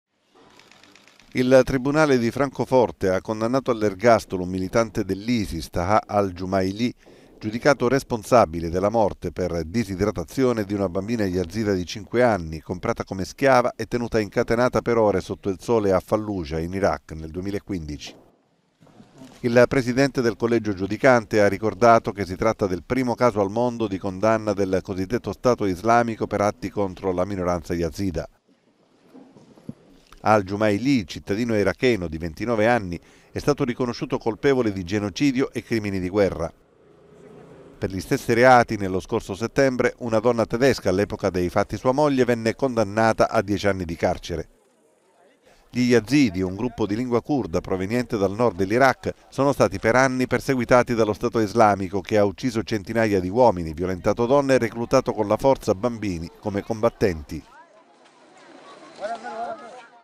Germania, ergastolo a un miliziano dell'Isis
Giudicato colpevole di crimini di guerra e contro l'umanità per la morte di una bambina yazida di 5 anni comprata come schiava